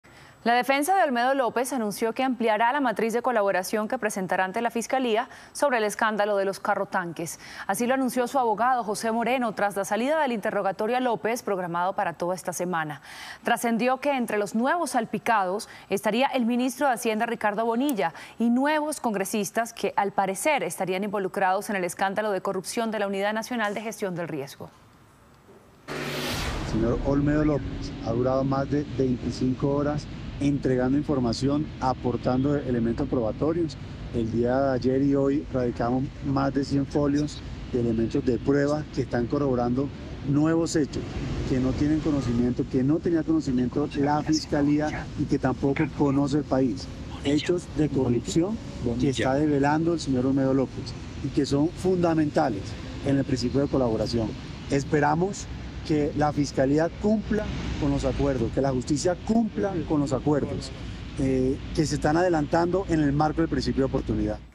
Olmedo López ampliará la matriz de colaboración ante la Fiscalía: salpicaría al Ministro de Hacienda
El exdirector de la UNGRD Olmedo López incluirá nuevos nombres a la matriz de colaboración con la Fiscalía. Entre los salpicados estaría el ministro de Hacienda, Ricardo Bonilla, y algunos congresistas.